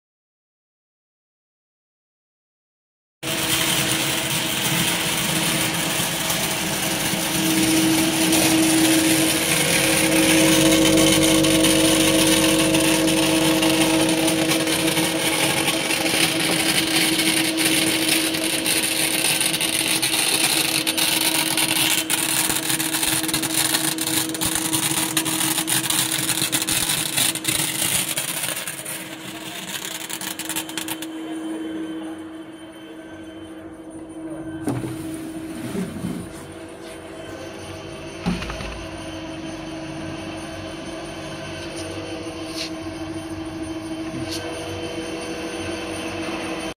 Belt Color Sorter Shrimp Skin Color Sorting Machine for Shelled Shrimp.
DREAM PLUS shrimp sorting machines sort cold water, warm water, brown or North Sea shrimps. The shrimp sorters detect and remove peel, shells, legs, eyes, tails and by-catch.Belt Color Sorter Shrimp Skin Color Sorting Machine for Shelled Shrimp.